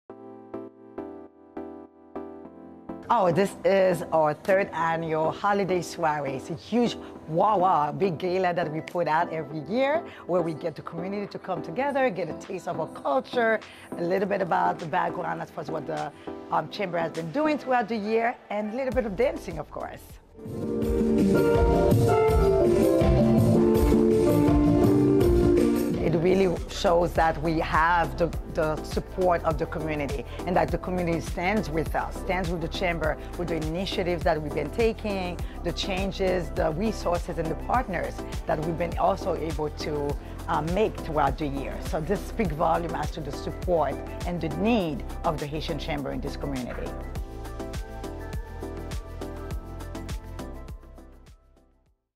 NEC and GHACC: Together We Grow!
An unbelievable evening with the Greater Haitian American Chamber Of Commerce-Orlando.

Orlando has become a major hub for Haitian American business, with a population of more than 40-thousand.